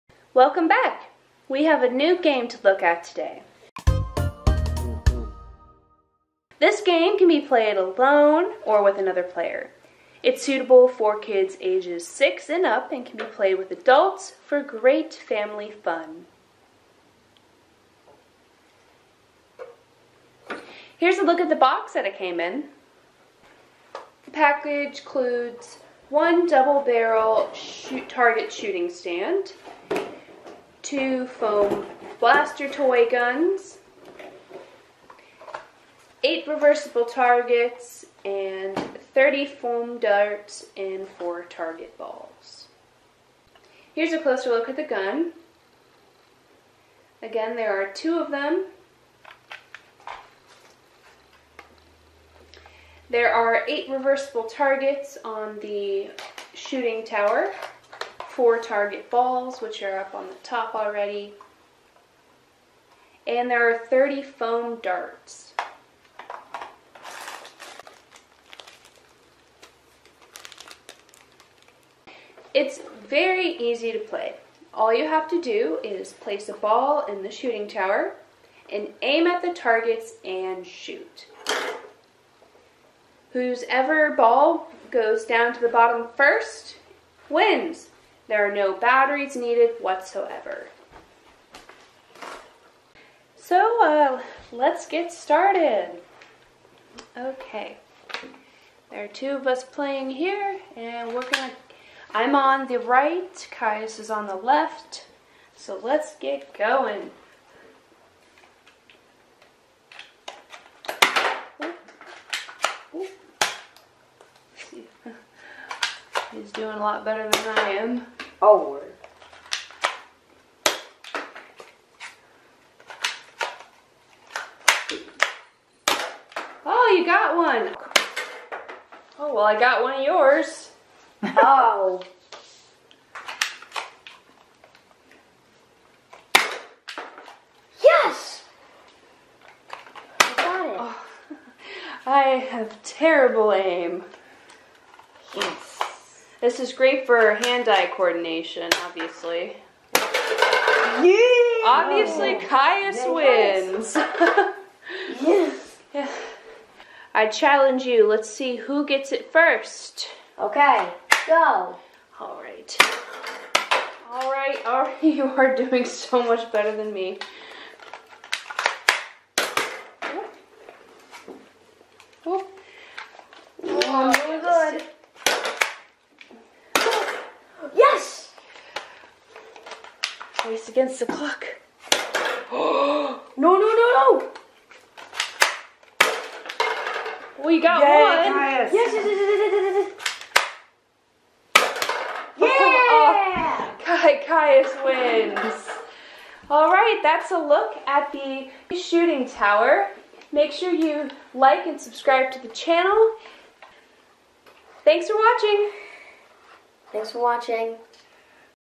Kovebble Shooting Game Toy💥Tower & 2 Foam Blasters 👈
Improve hand-eye coordination and have a ton of fun with the Kovebble Shooting Tower game.  This can be played indoors or outdoors and is suitable for kids 6 and up.